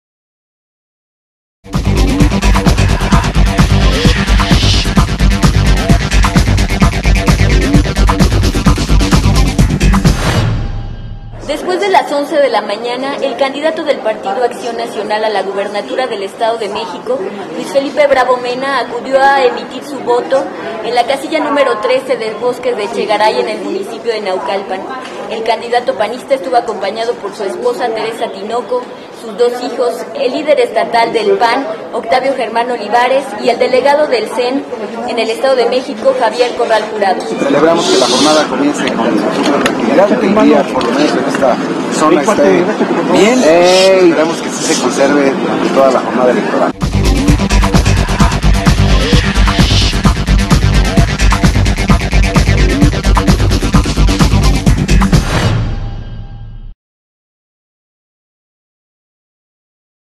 ELECCIONES 2011 VOTO BRAVO MENA. ESTADO DE MEXICO
El candidato del Partido Acción Nacional a la gubernatura del Estado de México Luis Felipe Bravo Mena emitió su voto alrededor de las 11 de la mañana. Televisa Estado de México.